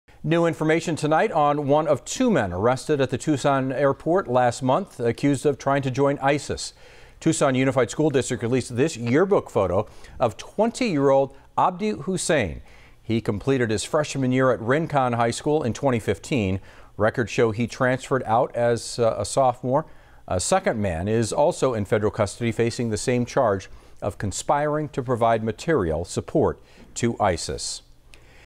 Tucson Unified School District release photo of accused terrorist
According to TUSD, he completed his freshman year at Rincon High School in 2015.